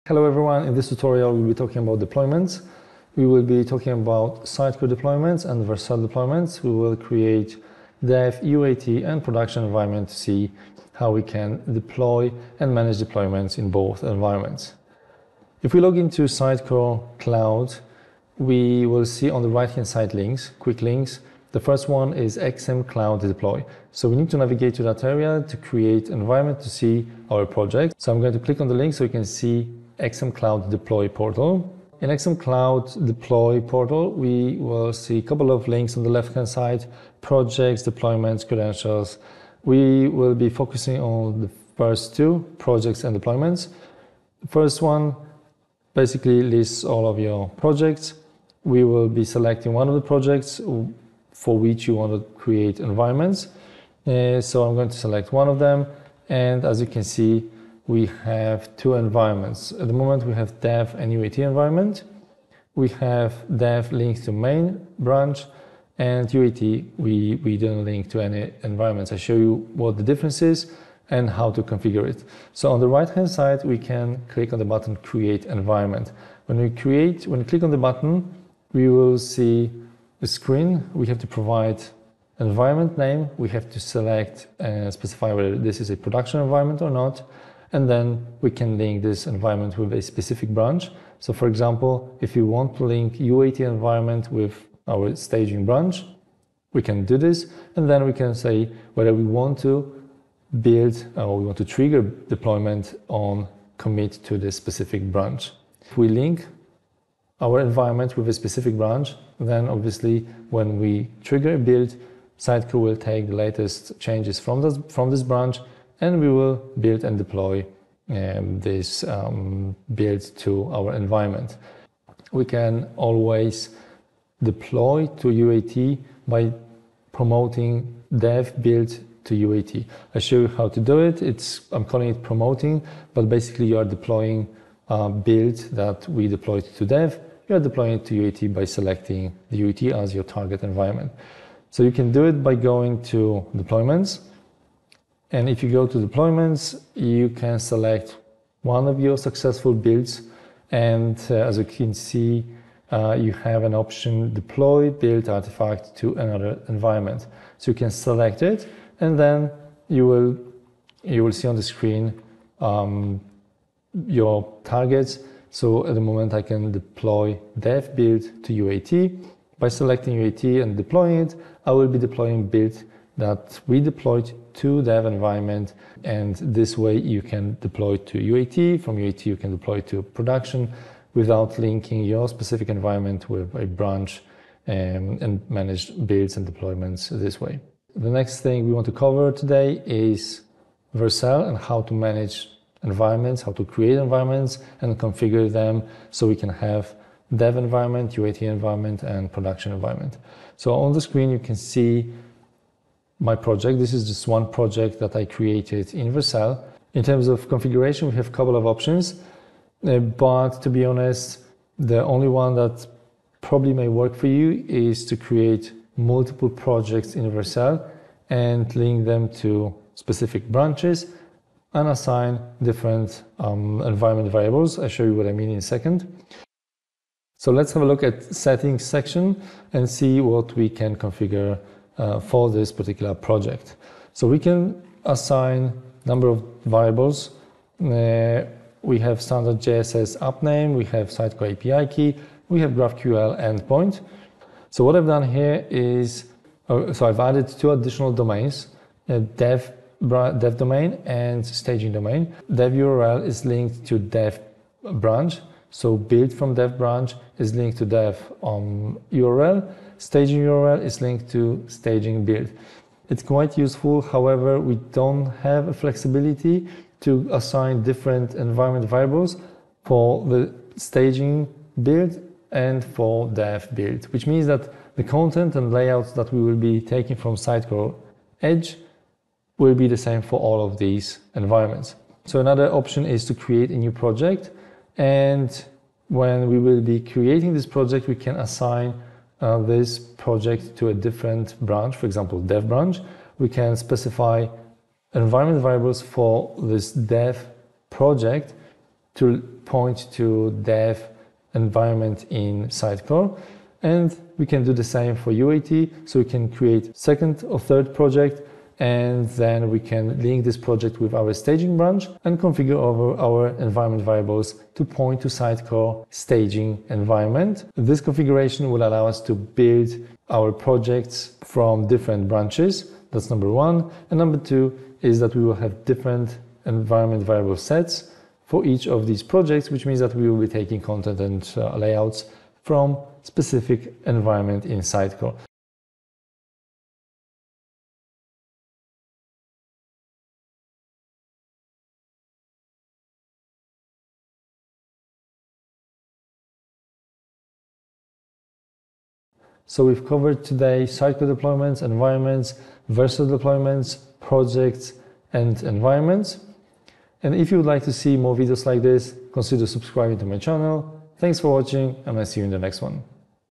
Deployments Sitecore Vercel - Sitecore XM Cloud
The video covers deployments in Sitecore XM Cloud and Vercel, including creating environments, linking them to specific branches, and managing builds and deployments.

It also explains how to configure multiple projects in Vercel and assign different environment variables for each project, allowing for separate content and layouts in each environment.